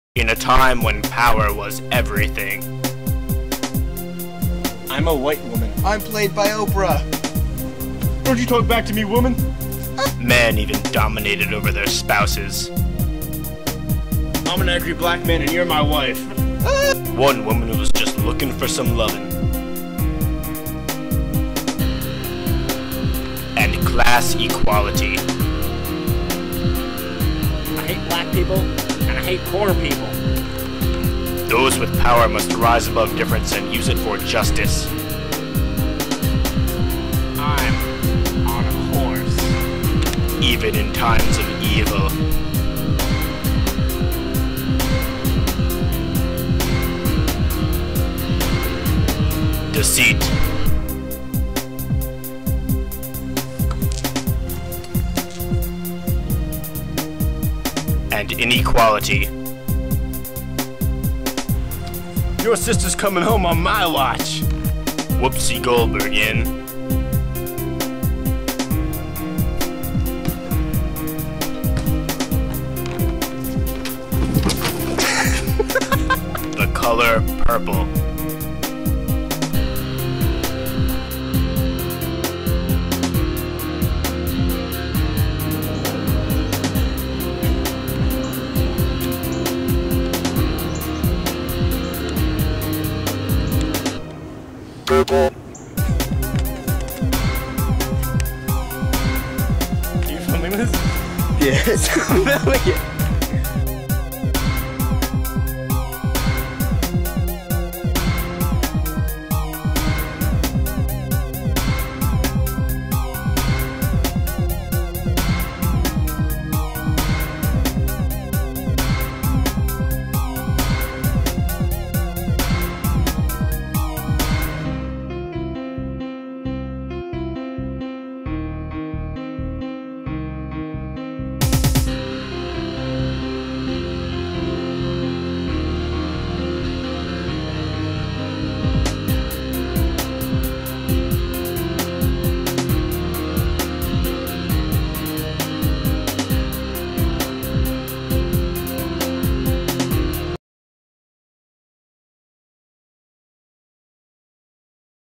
The Color Purple
we had to make a trailer for the color purple for english class and it turned out to be the greatest display of acting and musicianship ever. period. yeah we made the song i know its great.